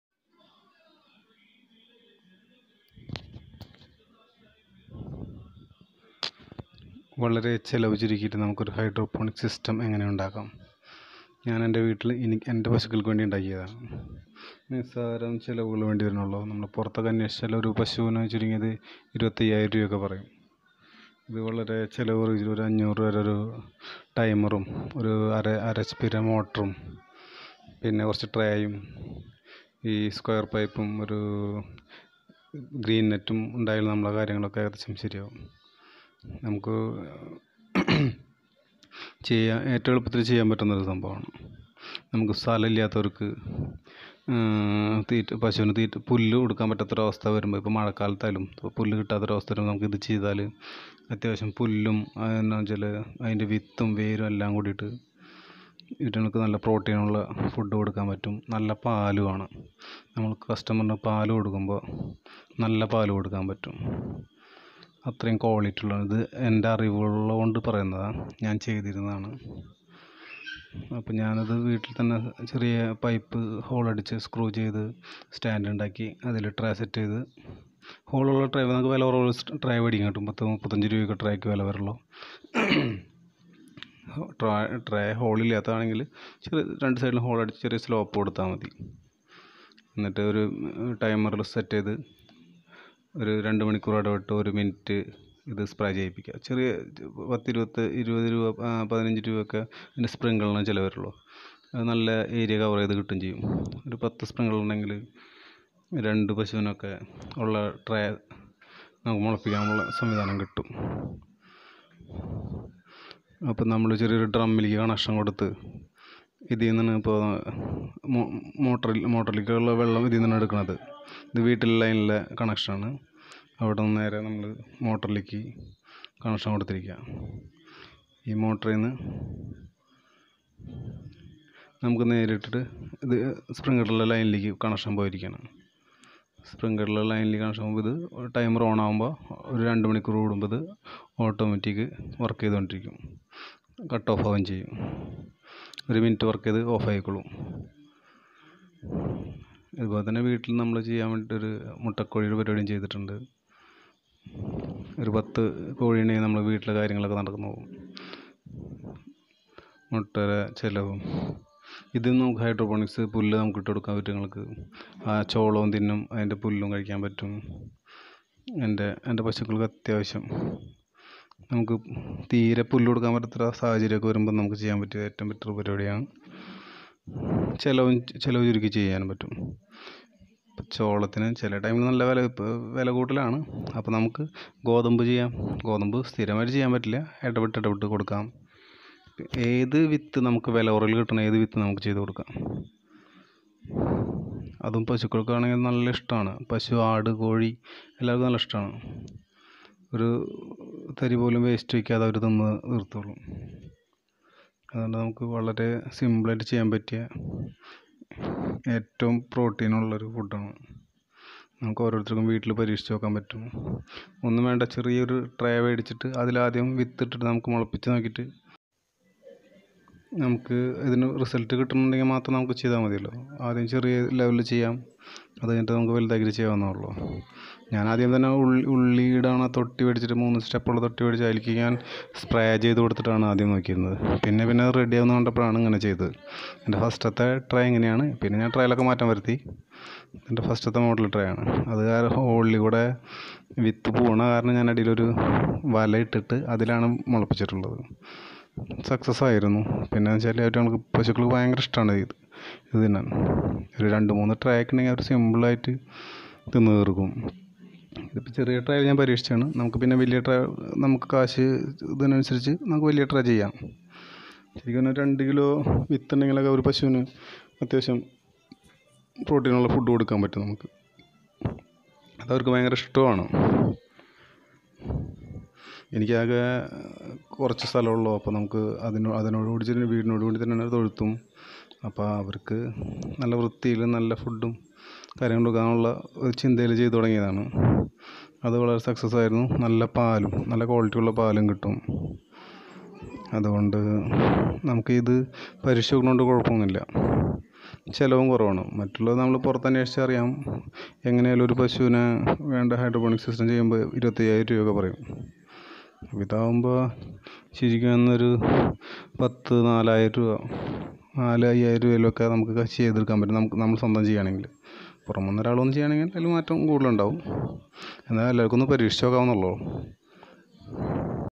Hydroponics farming ഹൈഡ്രോപോണിക്സ് ഫാമിംഗ്
വളരെ ചിലവ് ചുരുക്കി എങ്ങനെ ഒരു ഹൈഡ്രോപോണിക്സ് സിസ്റ്റം ഉണ്ടാക്കാം